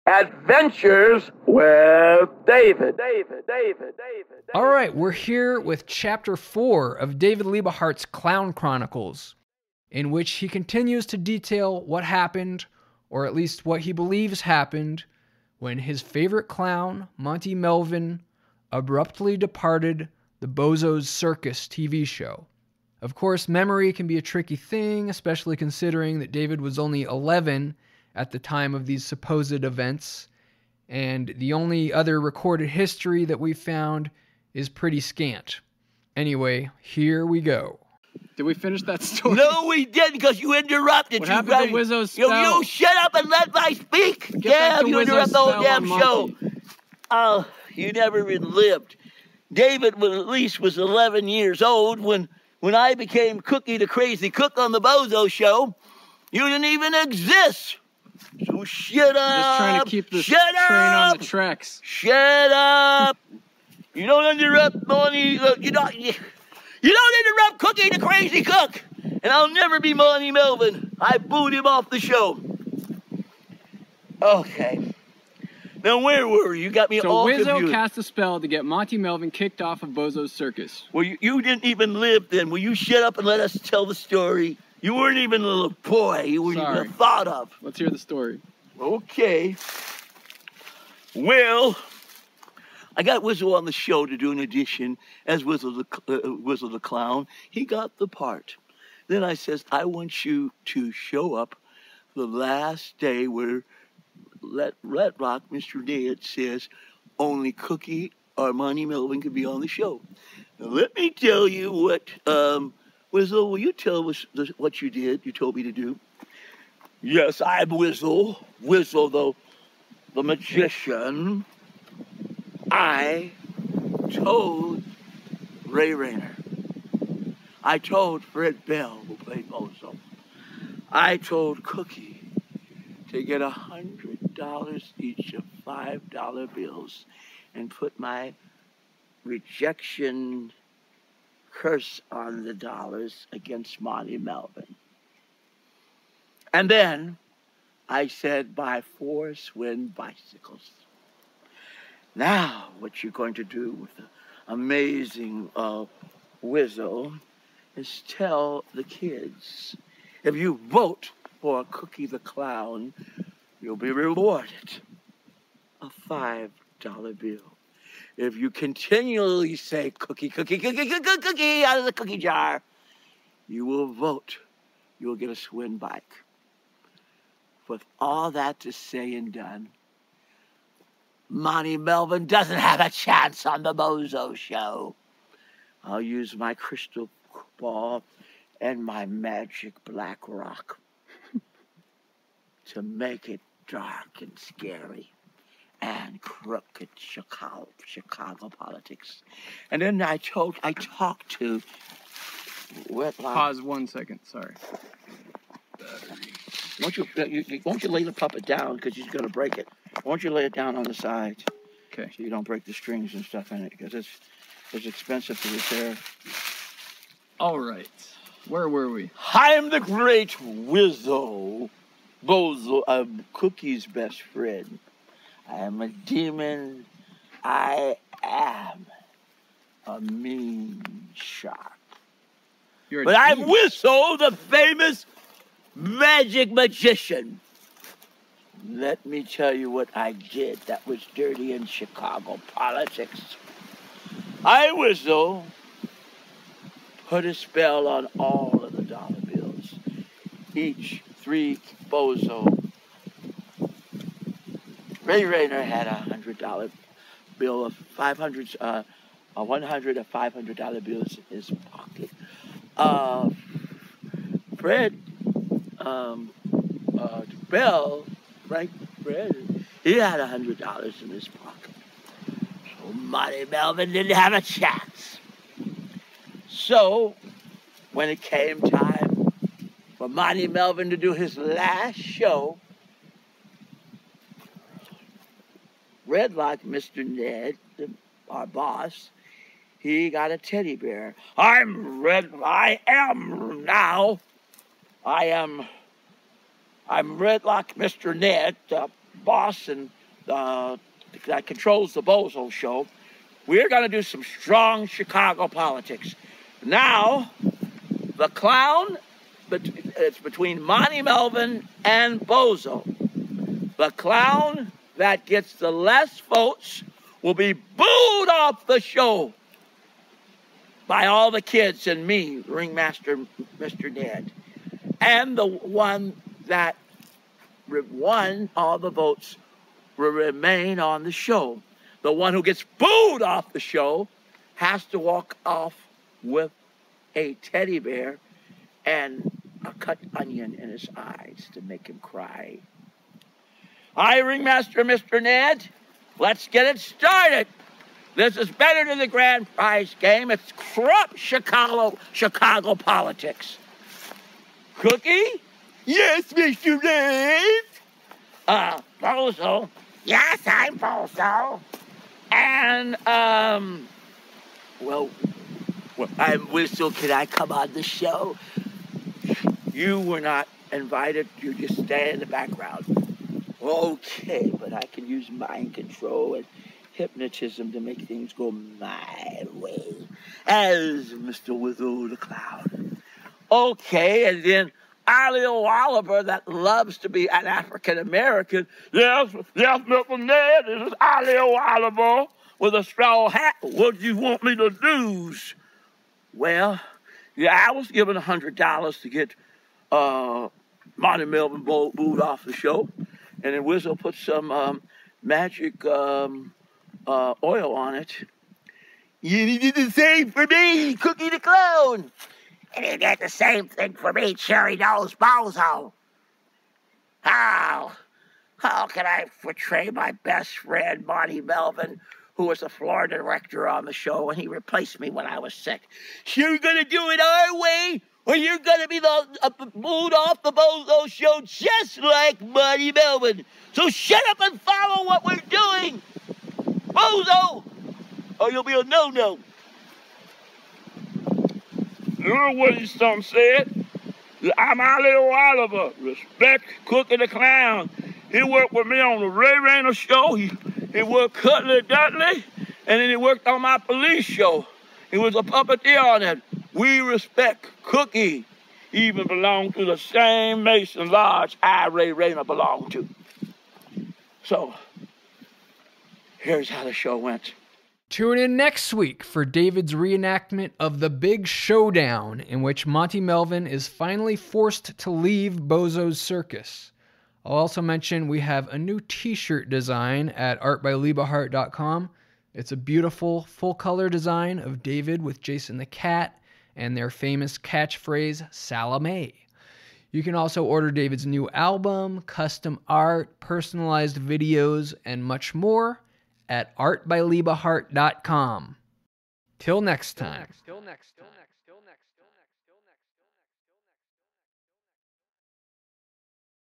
David Liebe Hart's Clown Chronicles #4
Chapter 4 of our 6-part series chronicling David's remembrance of, & obsession with, the great Bozo's Circus debacle of 1969, in which David's favorite TV clown, Monty Melvin, suddenly departed the show. David re-enacts the events as he recalls them, & speculates on the mysteries surrounding this bizarre tale of clowns, curses & conspiracy.